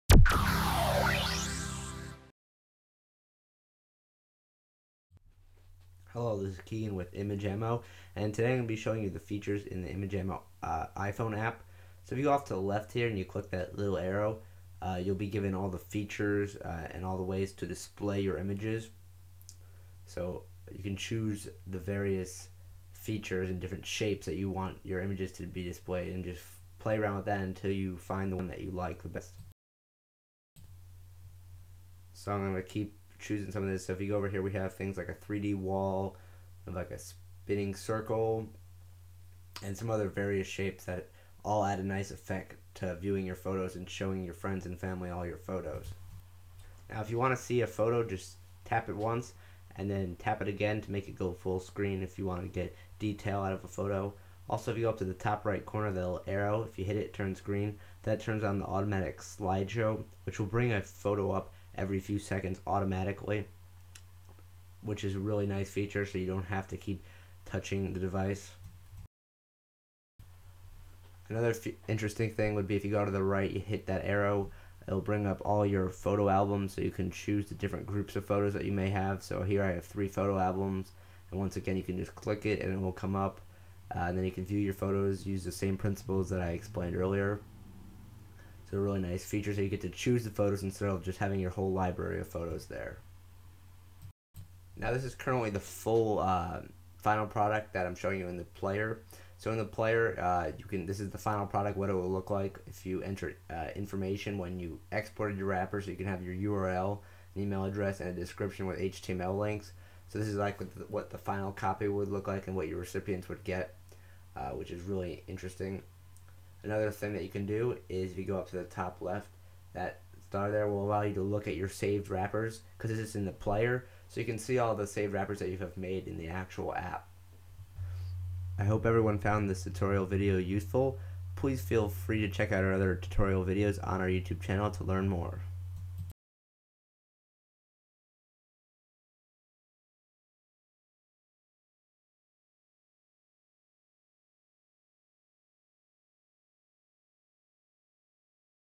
ImageAMMO Tutorial: iPhone - Key Features
Where can I get the enhanced version of ImageAMMO ...

ImageAMMO Description
View your collection of amazing photos in one simple to use application. ImageAMMO™ software takes your existing photos and displays them in a variety of 3D shapes. You will never go back to boring slide shows again! You can also use ImageAMMO to create wrapper files of your photos to share!

ImageAMMO Wrapper
Added the ability to export your display as an ImageAMMO Wrapper. You can then share this wrapper as you would any other document via email, flash drive, or any other medium. People you share wrappers with will need either ImageAMMO, or the free ImageAMMO Player to view your wrapper files.  For more information on ImageAMMO and Wrapper files please visit ImageAMMO.com.